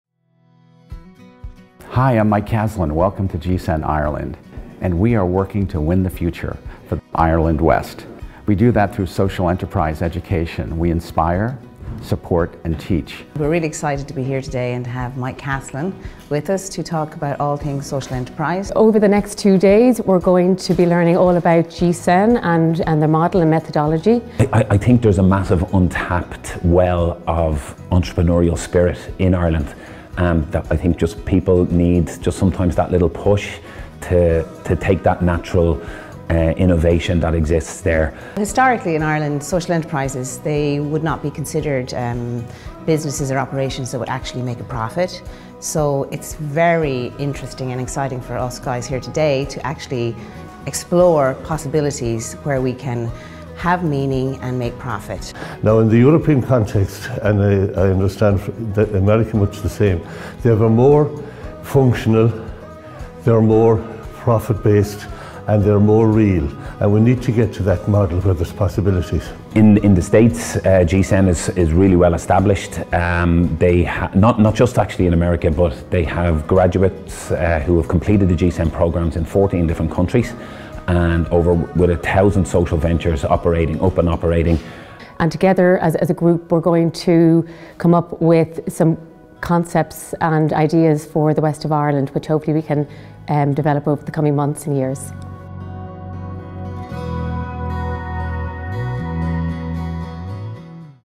GCSEN's Social Enterprise Incubator Launch in Mayo/Roscommon Local Area Service
In May 2024, the Mayo-Roscommon Local Area Employment Service (MRLAES) invited the GCSEN Foundation to assist in the Innovation and Design Thinking for Activation of a Regional Social Enterprise Accelerator/Incubator. This innovative Social Enterprise effort is dedicated to inspiring, teaching and supporting employee-owned Social Enterprises. Joining in will be Mayo North East LEADER Partnership Company, Atlantic Technological University (ATU) and Ulster University to help the region, “Win The Future!”